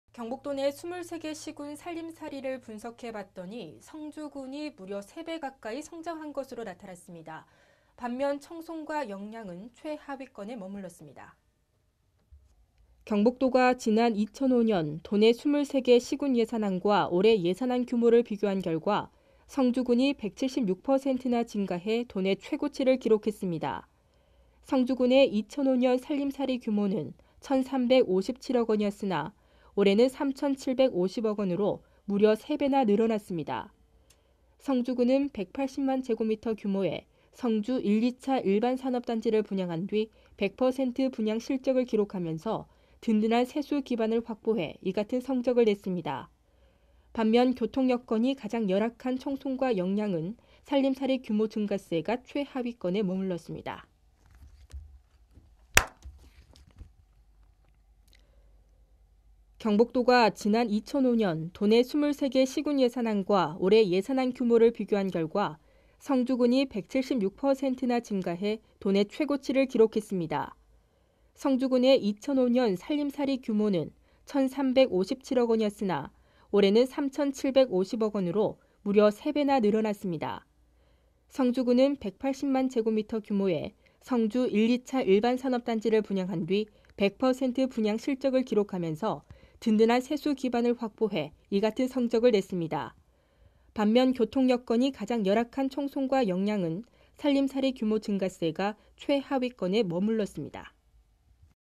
'성주군 곳간' 10년 새 3배 커져
경북도내 23개 시군 살림살이를 분석해 봤더니 성주군이 무려 3배 가까이 성장했습니다. 반면 청송과 영양은 최하위권에 머물렀습니다.

경북도가 지난 2005년 도내 23개 시`군 예산안과 올해 예산안 규모를 비교한 결과, 성주군이 176%나 증가해 도내 최고치를 기록했습니다.

성주군의 2005년 살림살이 규모는 1천357억원이었으나 올해는 3천750억원으로 무려 3배나 늘었습니다.

성주군은 180만㎡ 규모의 성주 1`2차 일반산업단지를 분양한 뒤 100% 분양 실적을 기록하면서 모두 78개의 기업을 불러들이는 등 든든한 세수 기반을 확보해 이같은 성적을 냈습니다.

반면 교통여건이 가장 열악한 청송과 영양은 살림살이 규모 증가세가 최하위권에 머물렀습니다.